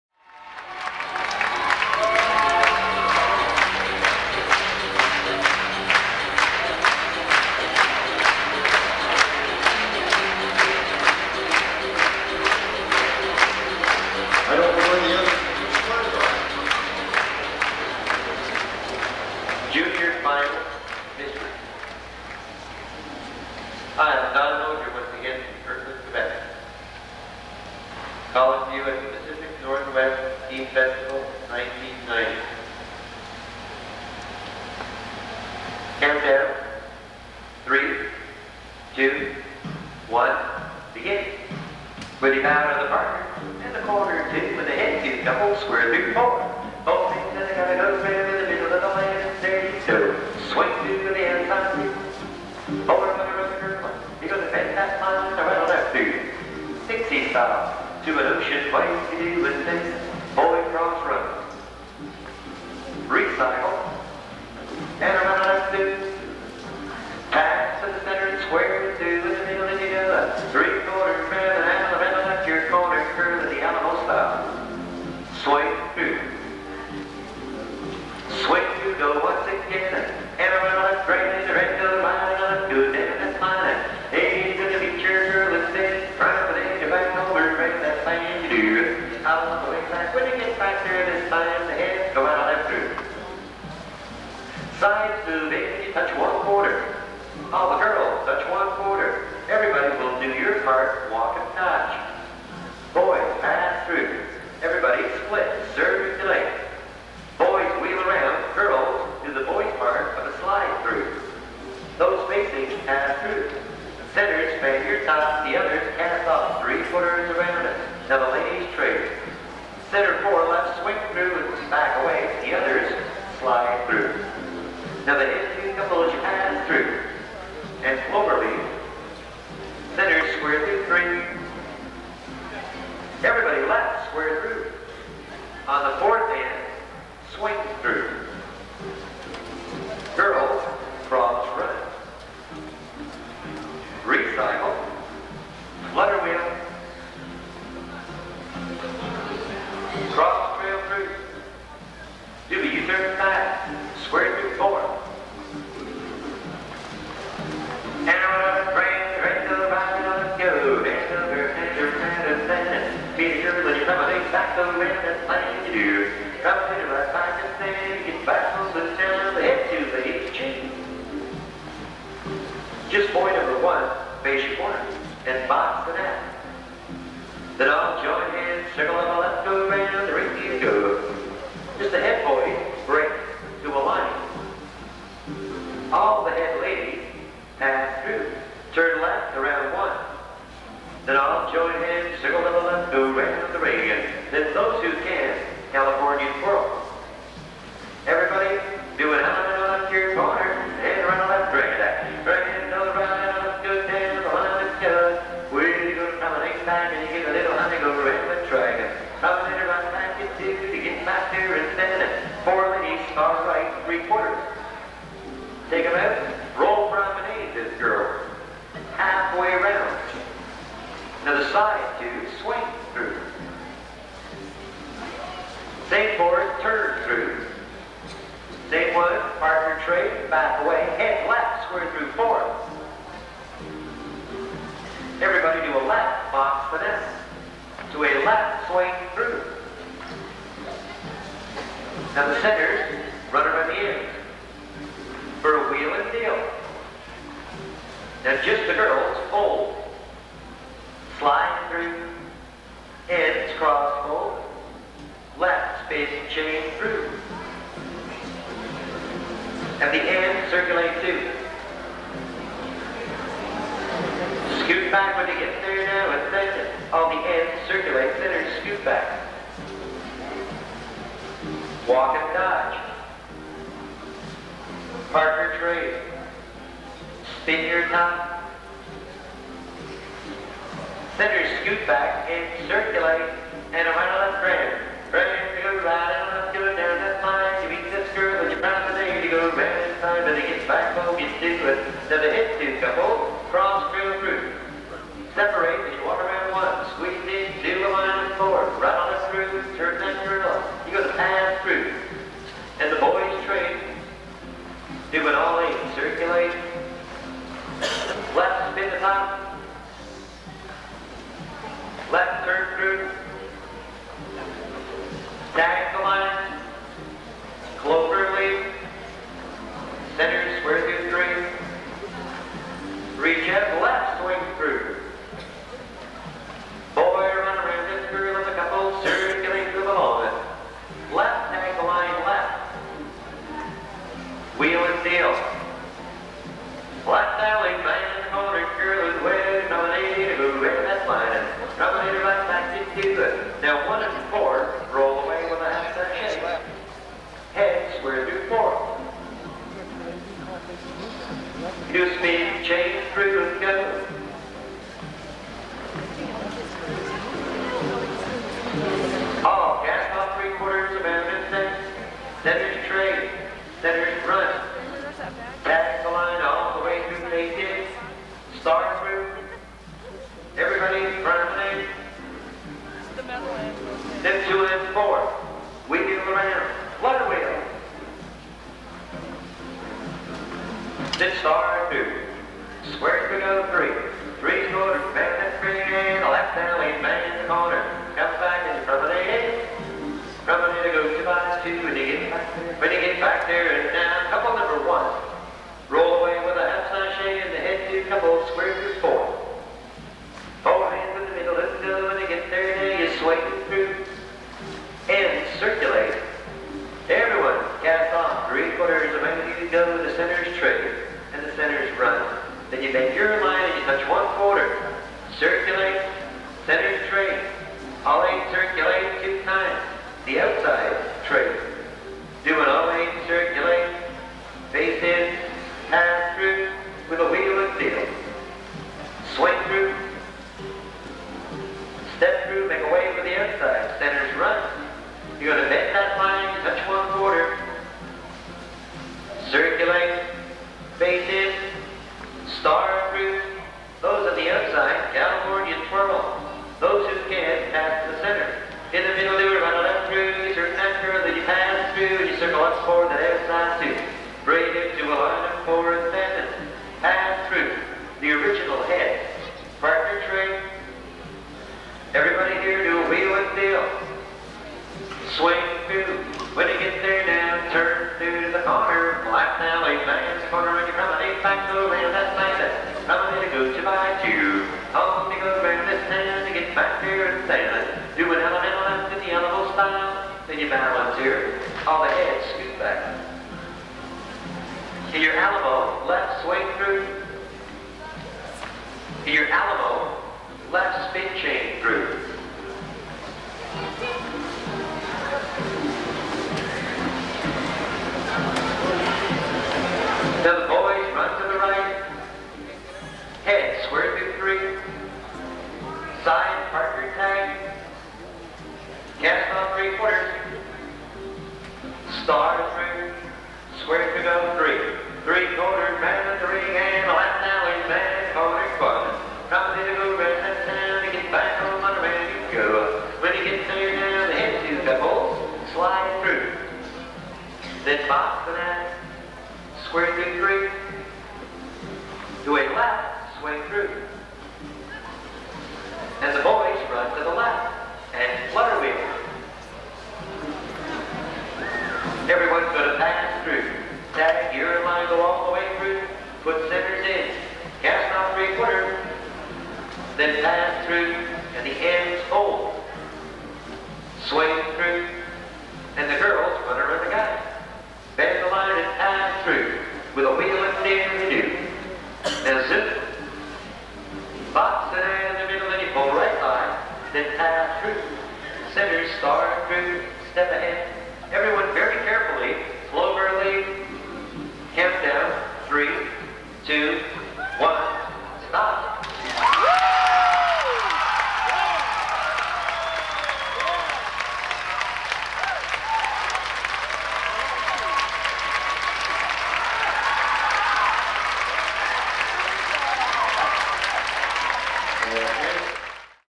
1990 PNTSDF Junior Final
1990 PNTSDF Junior Champion Square dancing the final mystery